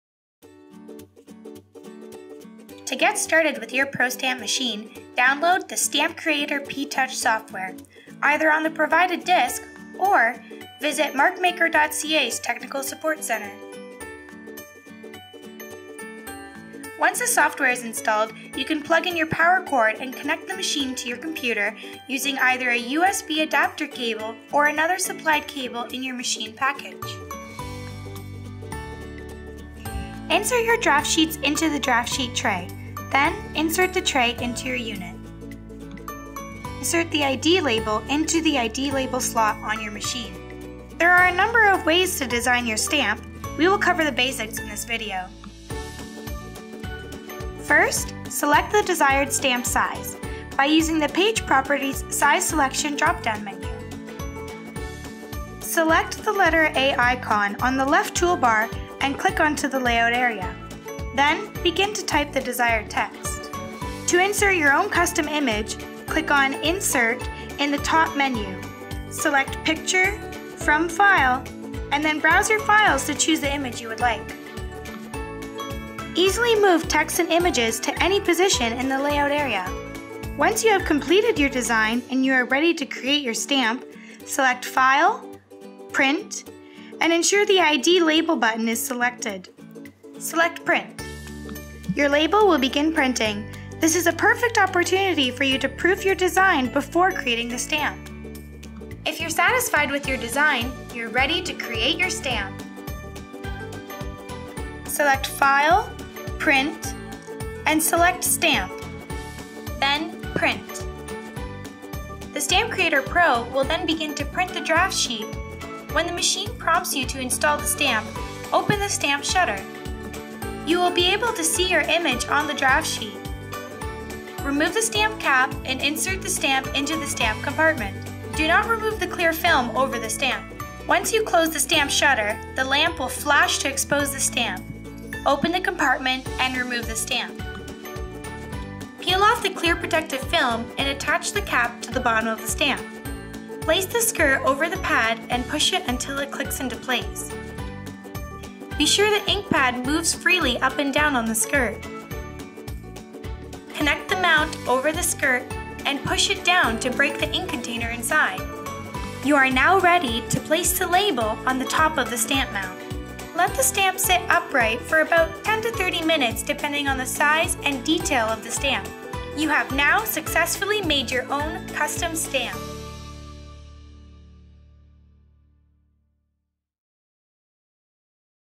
How to Make Custom Rubber Stamp in Minutes - Stampcreator Pro
The Stampcreator Pro machine is a printer that allows anyone to make custom rubber stamps (Prostamps) in only three minutes! With the ability to make text, graphics, signatures, and even photos into stamps. Prostamps are pre-inked and re-inkable. Up to 50,000 impressions before re-inking! Almost any design can be made into a premium professional stamp - in-house, anytime. Options include 11 different sizes.